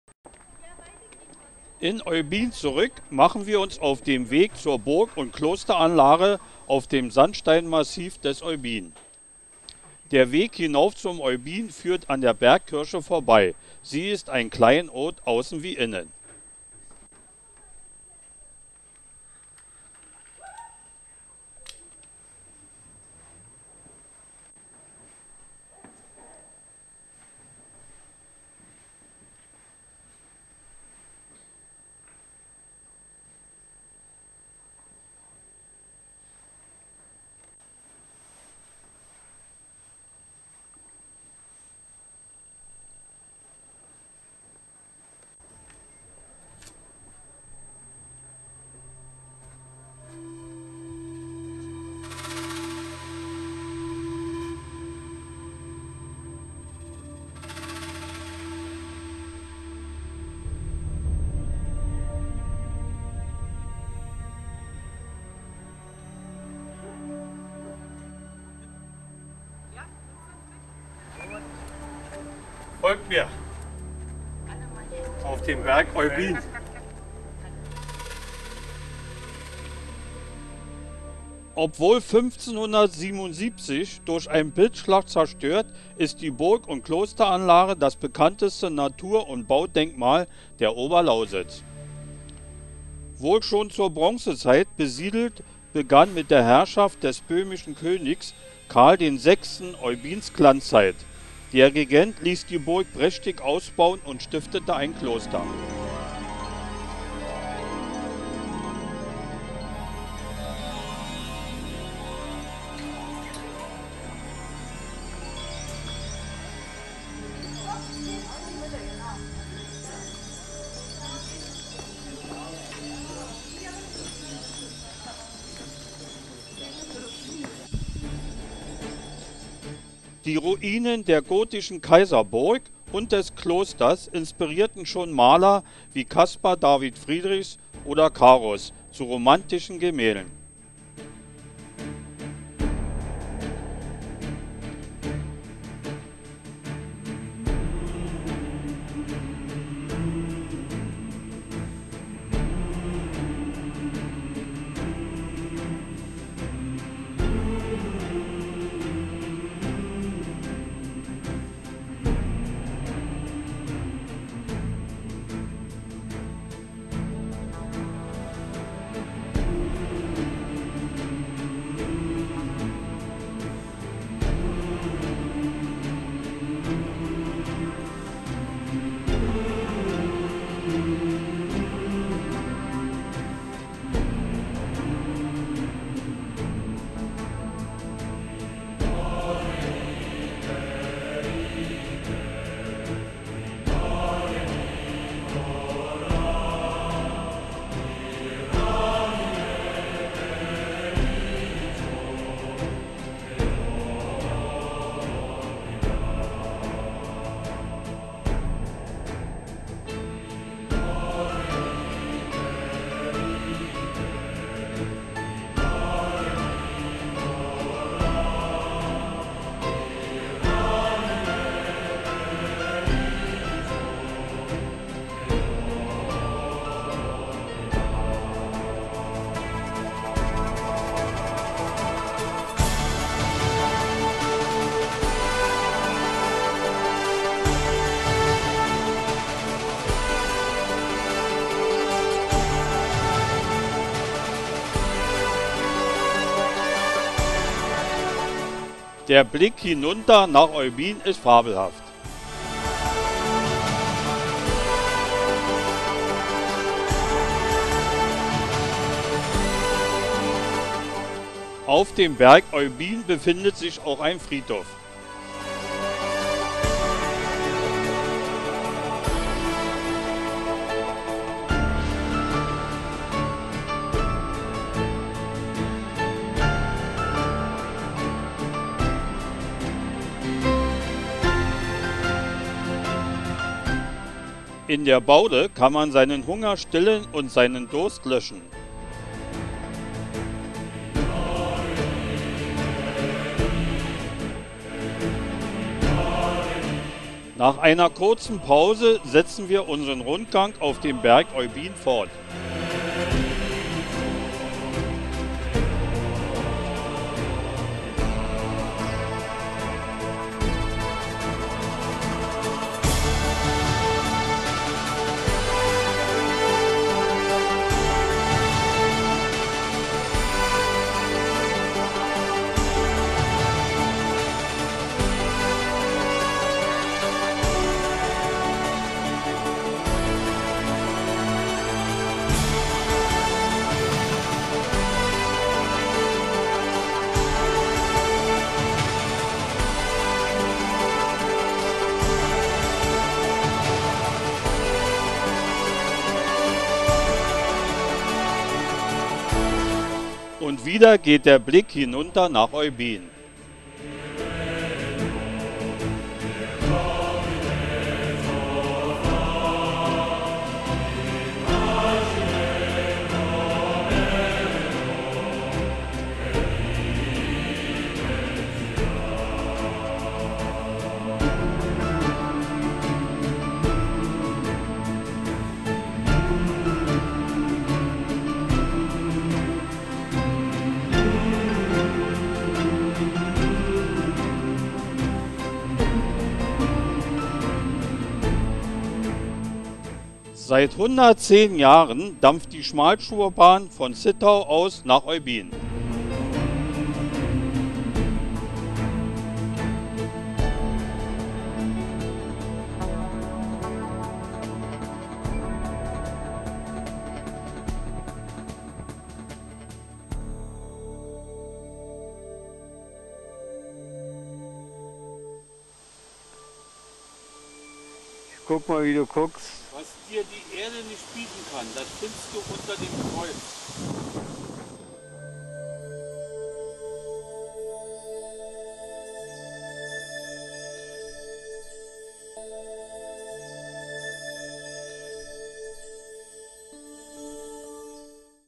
Berg Oybin
Fels Oybin bietet eine mystisch - märchenhafte Kulisse.  Die Burg - und Klosteranlage auf dem markanten Plateau zeugt bis heute von der einstigen Pracht und Herrlichkeit.